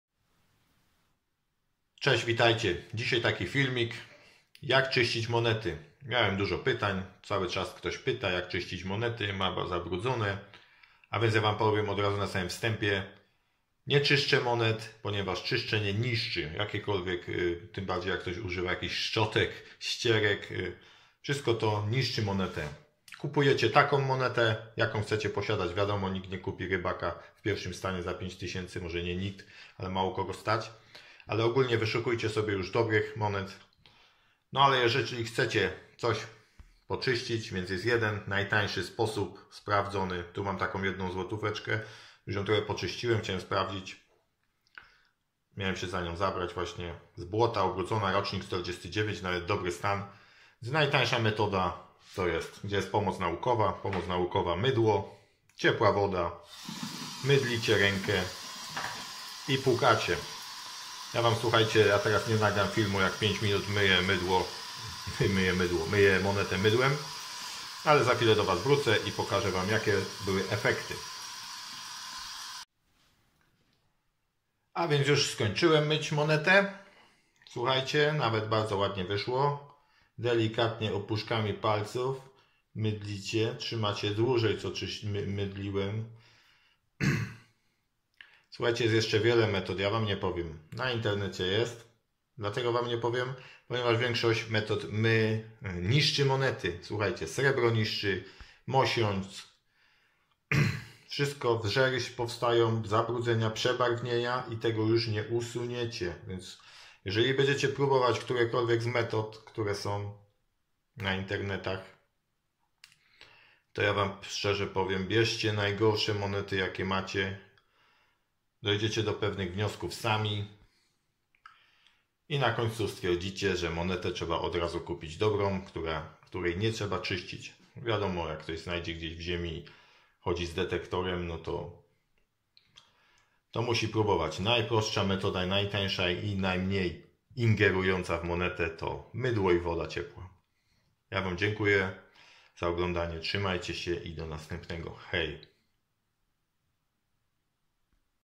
Czyszczenie Monet kolekcjonerskich, najtańszy domowy sposób, cleaning coins.
Witajcie, jestem zwolennikiem nie czyszczenia monet, powoduje to nieodwracalne zmiany i obniza wartośc monet, zapraszam na film.